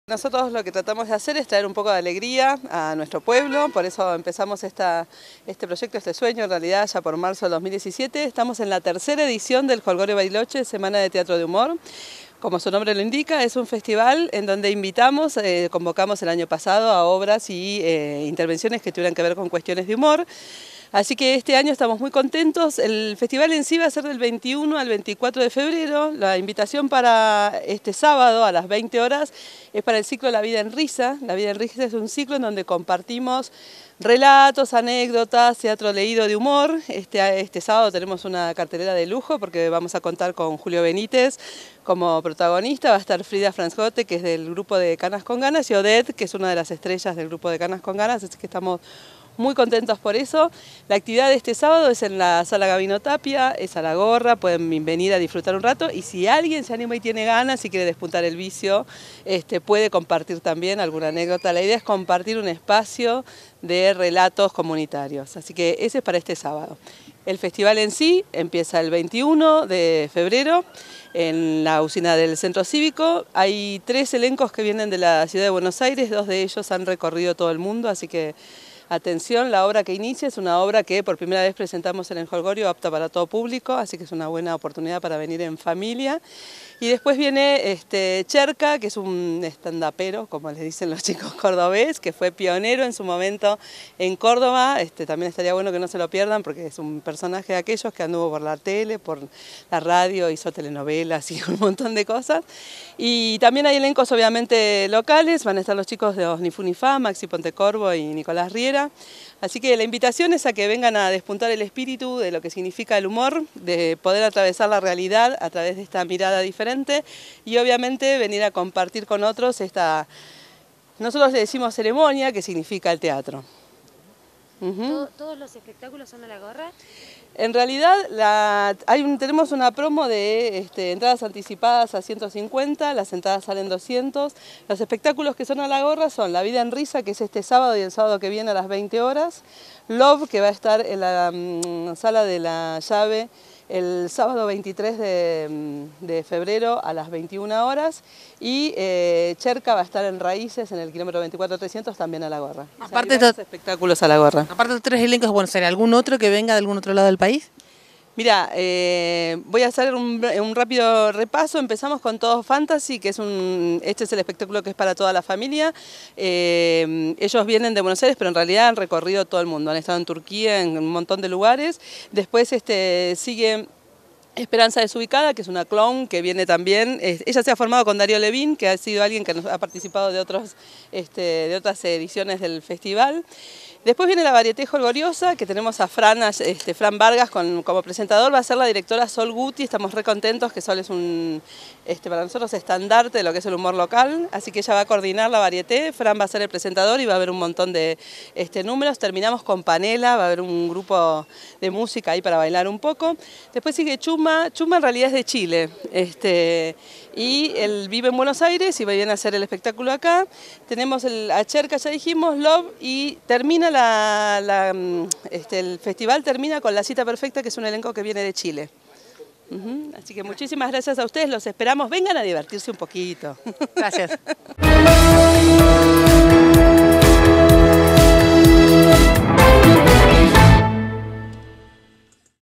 INVITAN A “LA VIDA EN RISA”, EN EL MARCO DE UNA NUEVA EDICIÓN DE “JOLGORIO BARILOCHE”
Este sábado desde las 20 llega el 3º encuentro de Jolgorio, “La Vida en Risa”. Se trata de un encuentro para distenderse y reírse en la Sala Gabino Tapia ubicada detrás del SCUM. Escuchamos la palabra de Silvia Pérez Sisay.